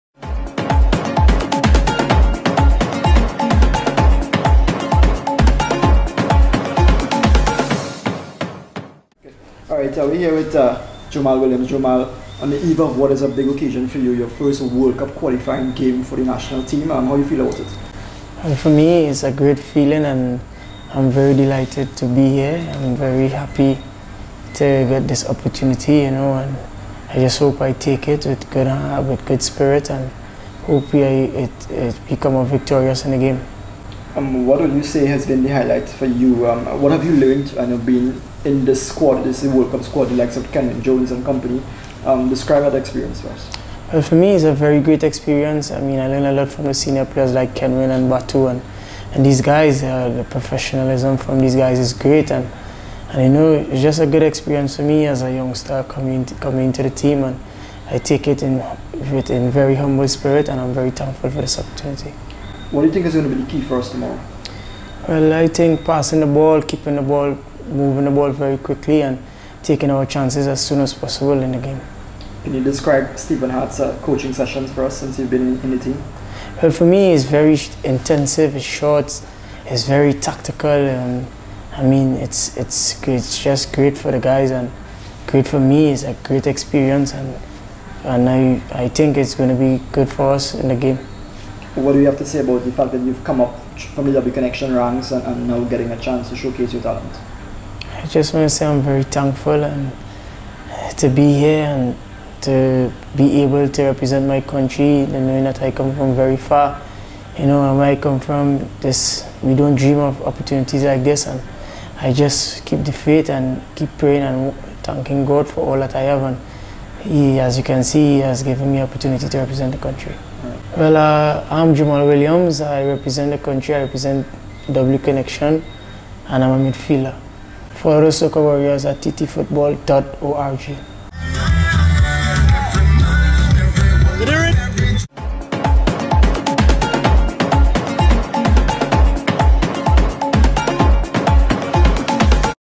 Jomal Williams talk about his experience with the 'Warriors'
DirecTV W Connection midfielder Jomal Williams talks about his experience with the Trinidad and Tobago National Men's team - TTFA Media TV